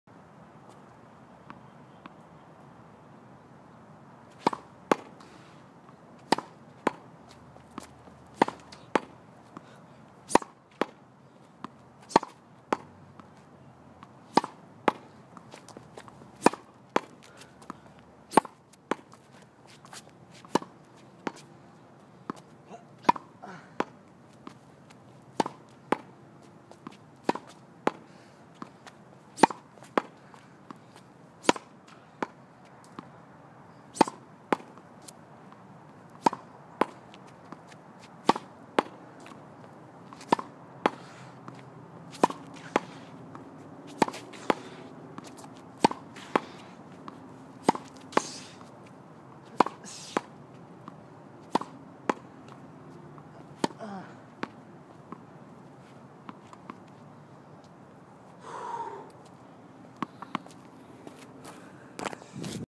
Hittin against the wall
Playing a little tennis against the wall during my break at work haha